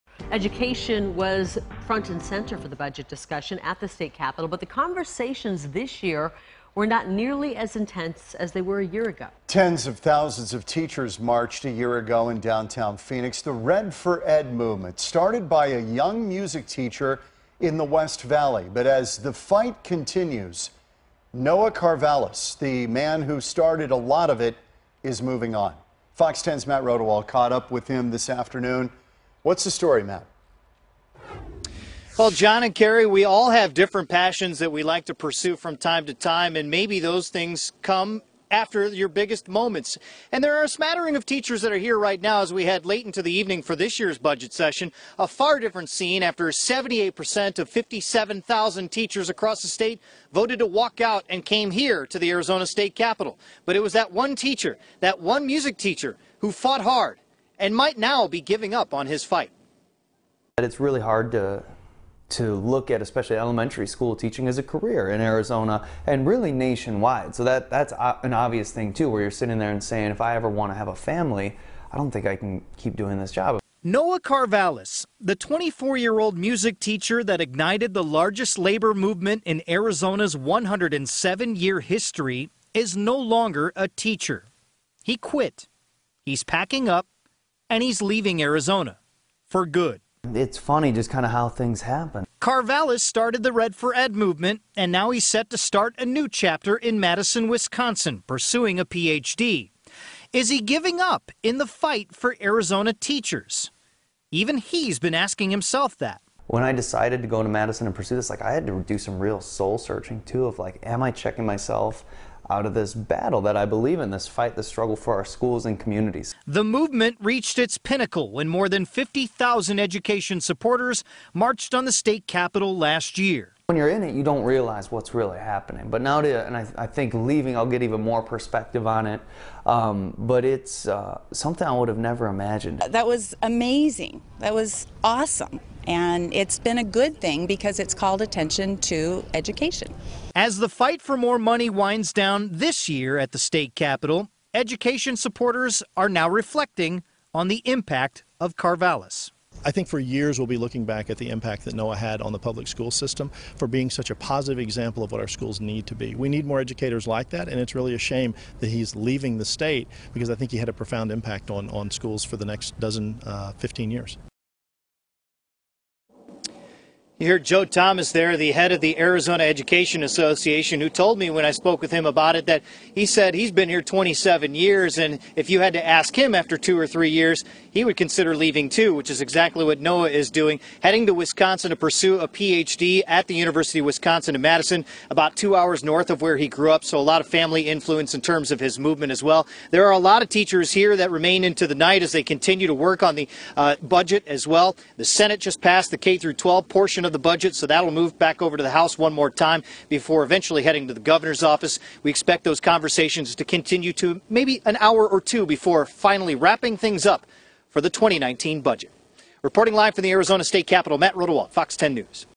RedforED activist Noah Karvelis to leave Arizona for Wisconsin 9pm
The 20-something music teacher who started a grassroots moving is going back to school in Wisconsin. This was an exclusive on Fox 10